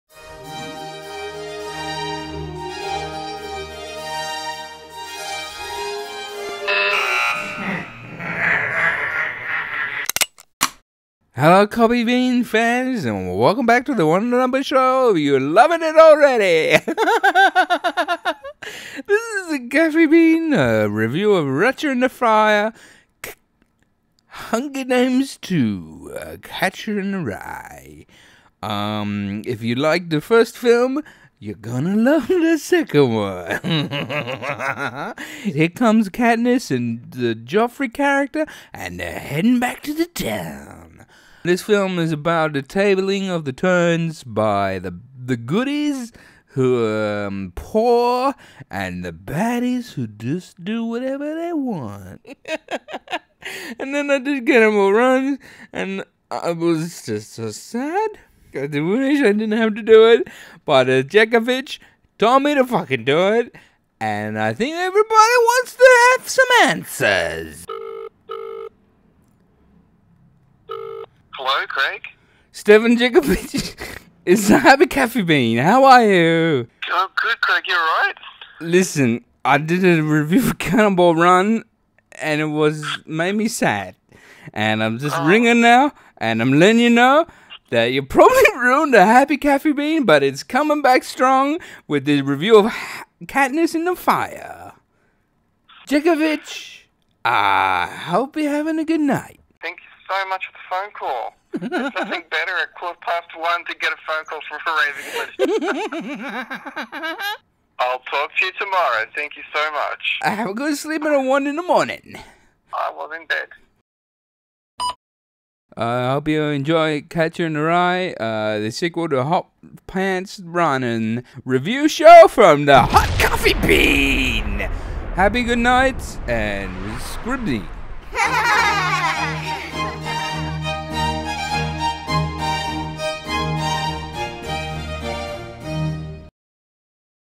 Hunger Games 2 : Catching Fire Review (2013) - Happy Coffee Bean
Heren is what I think  of the nRe Catcher IN they Fire Movei fropm the hunger games series. I also gollowed up on the guy called Stevo KJepkovic who is a subscurber and asked met o to review THe CANONBALL RUN last week- but it made me sad/ So i though I wopulod dedicate this review to his memory and hisd family. Love and hope you enojyo the revioew .
 Coffe
 N